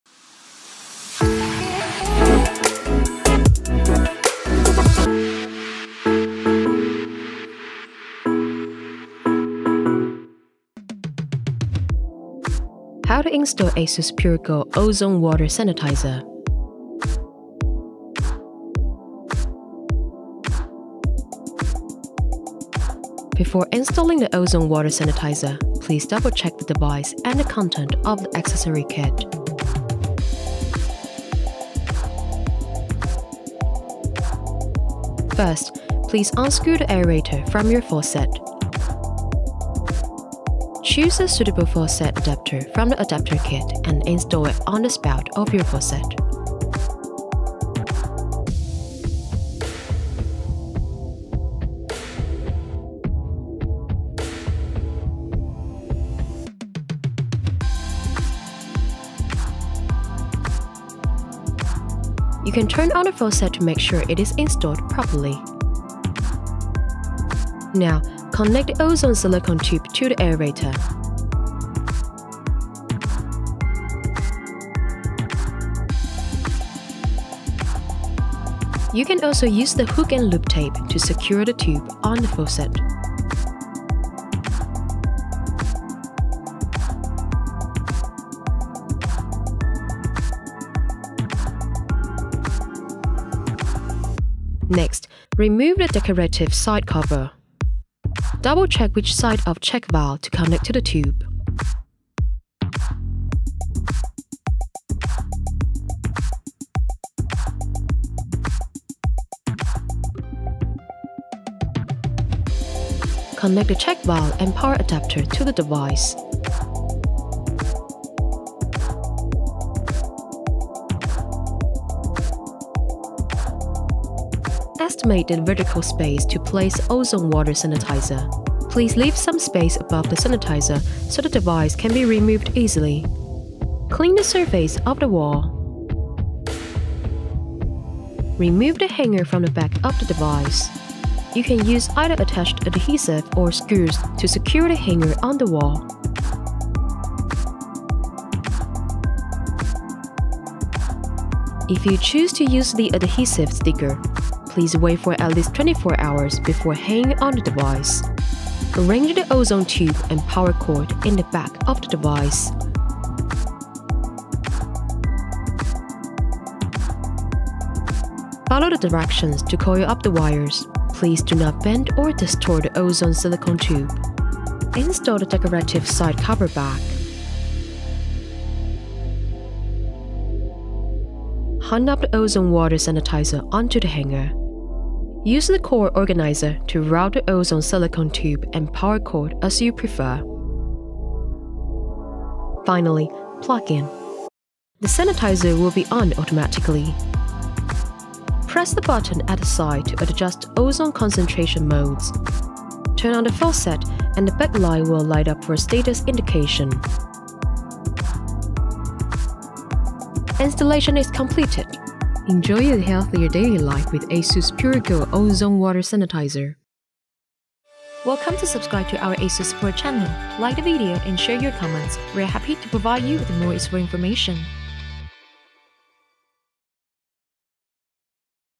How to Install ASUS PureGo Ozone Water Sanitizer | ASUS SUPPORT
ASUS PureGo OW100 is an ozone generator that provides users with ozone water right out of the tap. It’s a quick, low-cost, and Eco-friendly way to disinfect and clean the home.

Effective: 99.9% antibacterial protection with lab reports, a built-in safety protection mode automatically stops ozone generation after 20 minutes of continuous use, Eco-friendly with no need for consumables
Ease of Use: Easy to install within 5 mins and intuitive to use, conveniently switch between tap water and ozone water and adjust concentrations of ozone water as needed.

Aesthetic Design: Elegant design in Pure White, concealable wires and fasteners to ensure a neat setup in any kitchen.